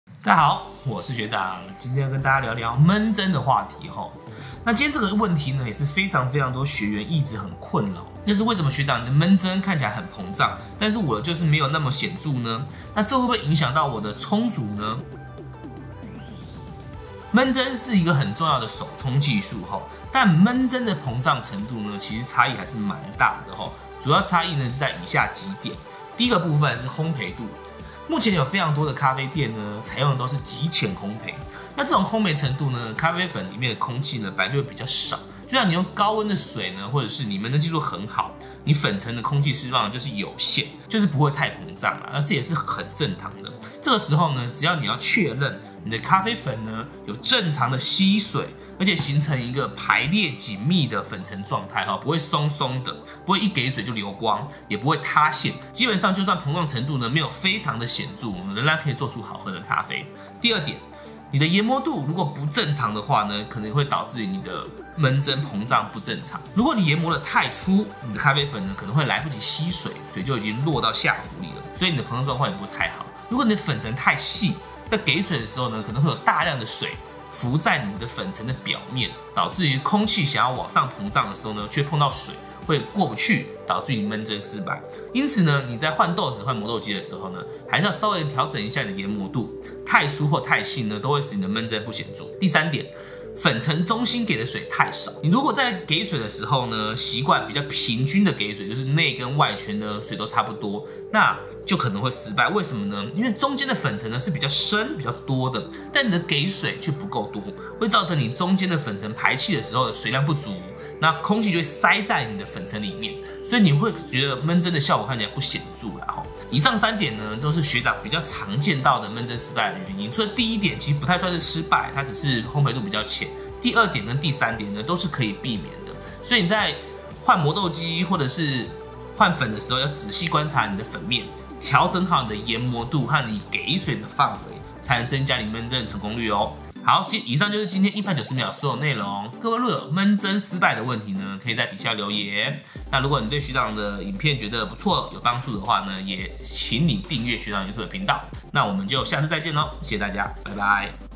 硬派90秒_#28_悶蒸膨脹問題
用90秒的時間帶給你最硬派的咖啡知識，硬派90秒開始！

為什麼有時候悶蒸的時候咖啡粉好膨脹
跟剛出爐的麵包一樣
但是有時候好像沒什麼反應
很多人跟學長說豆子都不膨脹
擔心影響風味

是不是這樣呢？
就讓學長來跟你說